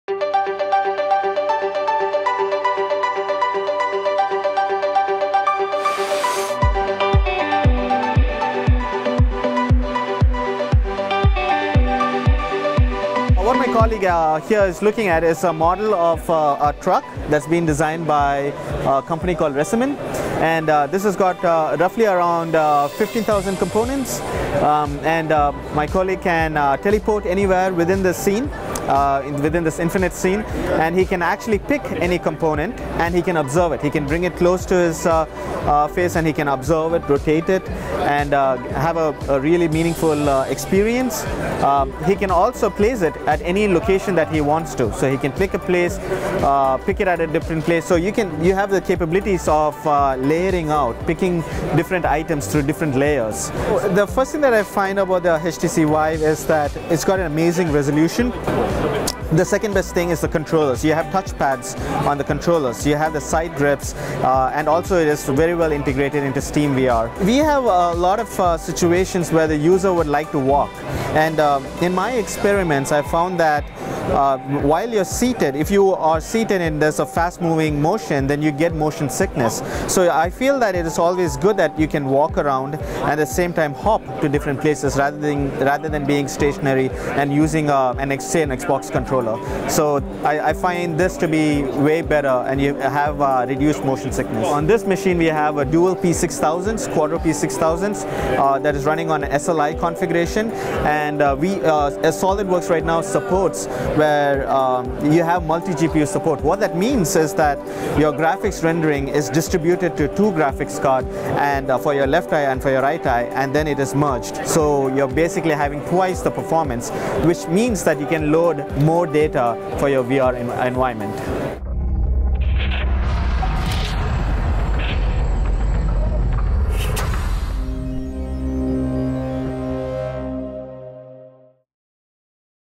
Dassault Systemes Shows of the Power of VR at SOLIDWORKS World 2017
Siddarth Palaniappan of Dassault Systemes gives an overview for VR powered by two NVIDIA Quadro P6000 GPUs in SLI with the help of the HTC Vive. Learn more about all of our SOLIDWORKS graphics solutions